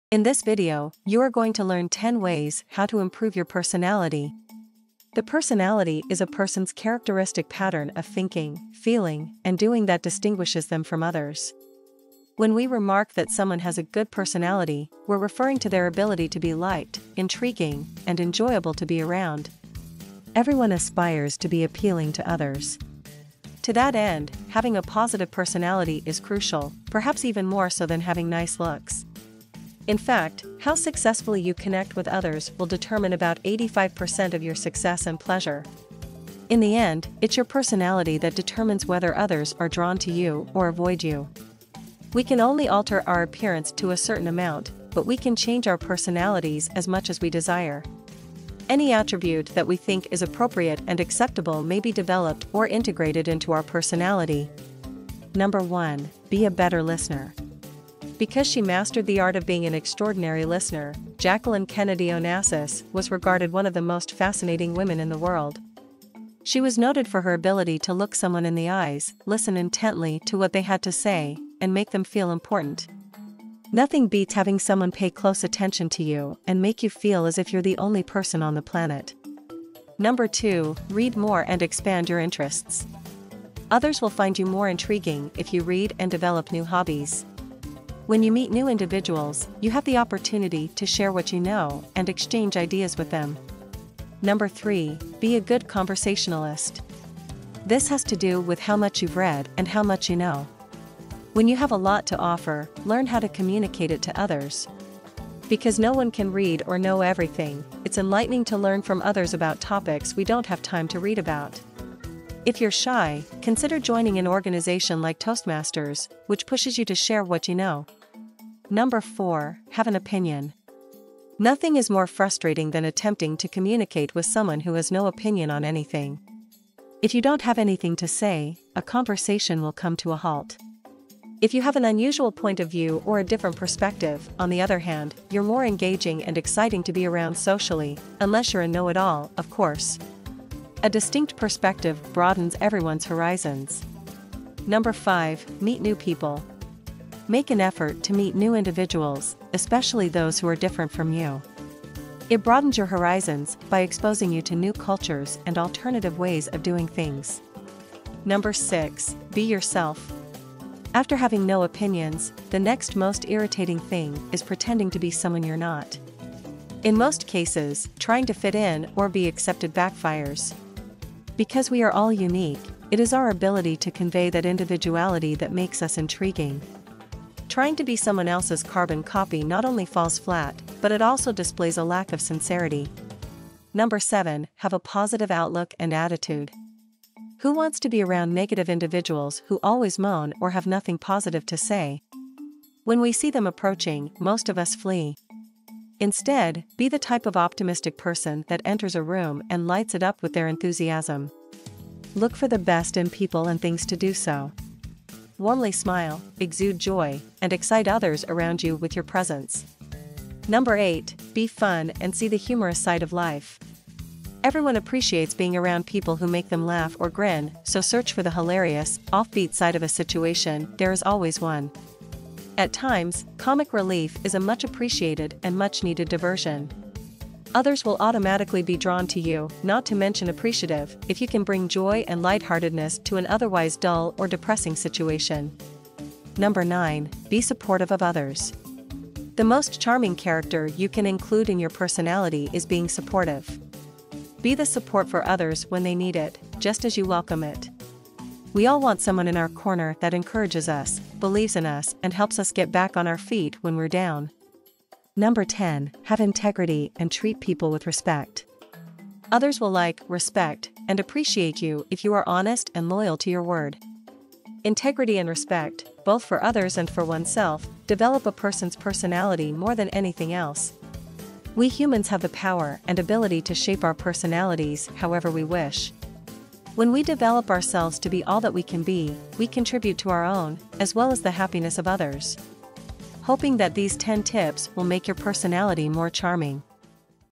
How to Be More Likeable? - 10 Ways to Improve Your Personality
Being more likeable is not about knowing how to be charming and charismatic, it's about being you. It's about understanding your personality type and maximizing the good qualities that make you unique.

But in this video, we are going to share with you 10 ways you can improve your personality today.